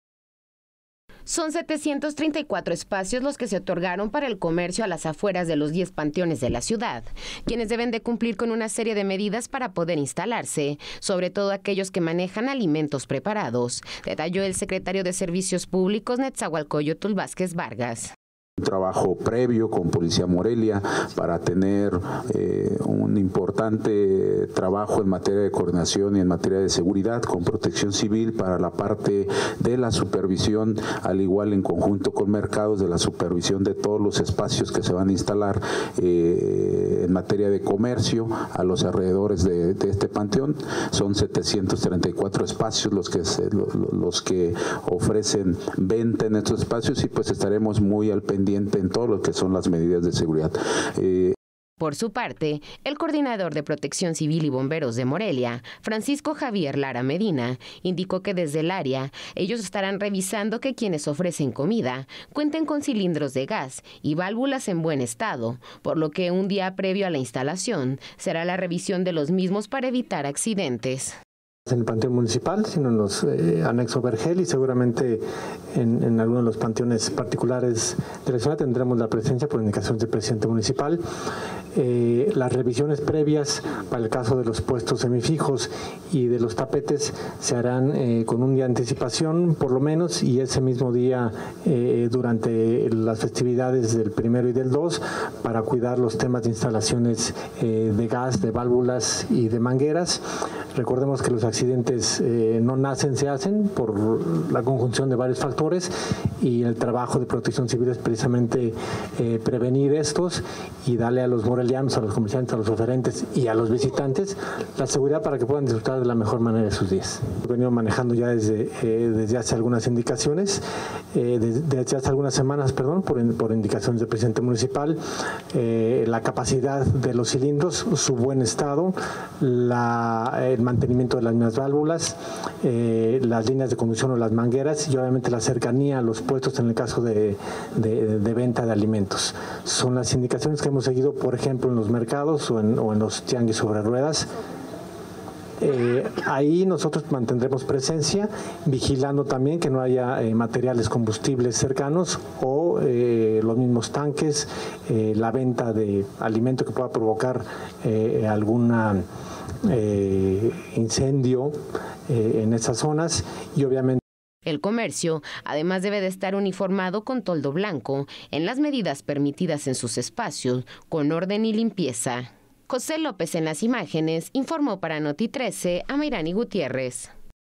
Supervisarán Comercios Qué Se Instalen En Los Panteones
Morelia 🔴 Se supervisará que todo comercio que se instale en los panteones de Morelia cuenten con medidas de seguridad, sobre todo los que venden alimentos.